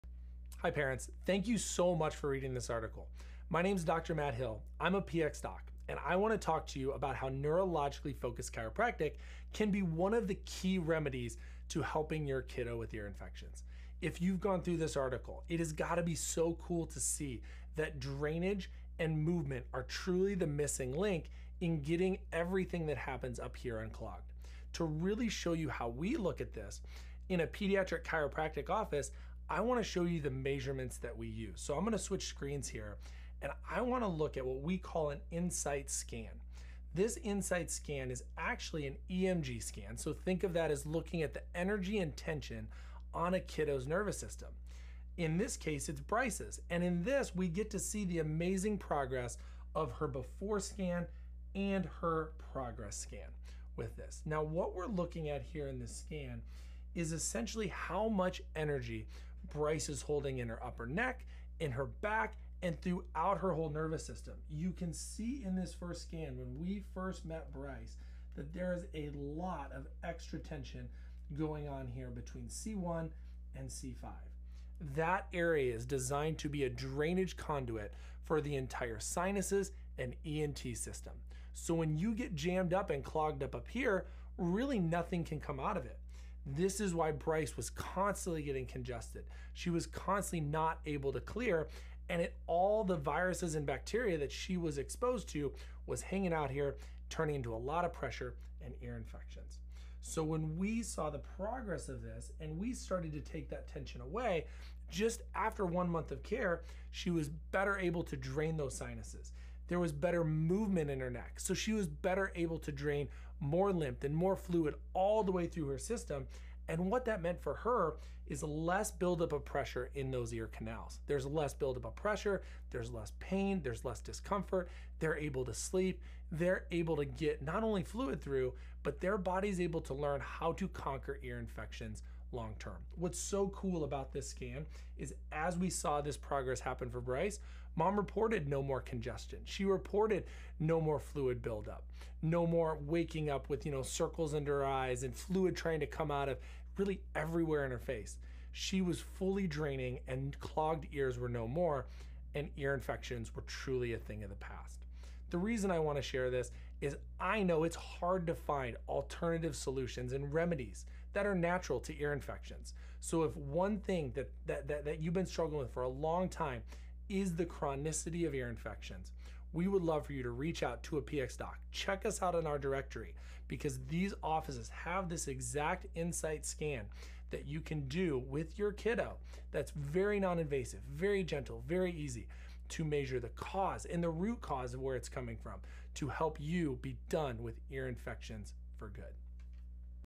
Natural Ear Infection Remedies For Kids - Bryce's Story
Like so many parents who are looking to take the natural, less invasive route dealing with ear infections 👂🏻 but are prescribed medications 💊 or tube surgery first, Bryce's mom was on a mission.

She was determined to find an effective option that would hold off ❌ surgery, as well as get to the root cause of Bryce’s chronic ear infections so that she could stop the seemingly endless cycle of pain, 😩 sleepless nights, 🥱 and antibiotics 💊 that she knew was wreaking havoc on her gut 💩 and immune system 🤧 long-term.

She was looking for natural remedies and drug-free options, and she knew she wouldn’t find those at her pediatrician’s office, 🏥 so she started looking into Neurologically-Focused Chiropractic Care.

And here was the good news we could share with her and parents everywhere – natural remedies for ear infections do exist! 🎉

These natural remedies have been around for a long time, and they help provide pain relief, 😅 reduce inflammation, 🔥 promote drainage, and over time work to not just help a child get past an acute ear infection but prevent further ones as well!

In order to best understand the role that natural remedies for ear infections play, we’ve first got to understand the root cause(s) of ear infections in the first place. That all starts with what we call the “plumbing,” 🪠 or drainage of the inner ear. 👂🏻